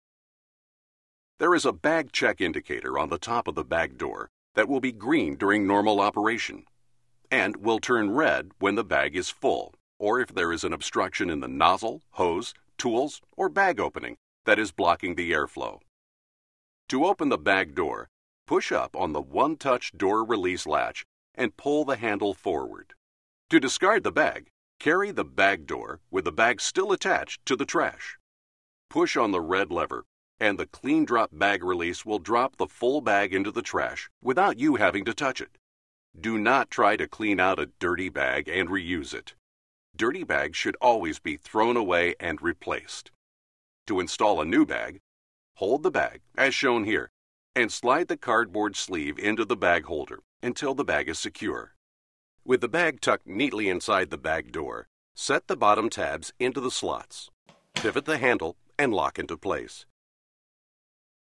Hoover T-Series Bagged Upright Bag Features UH30310
Bag indicator, removal, disposal and replacement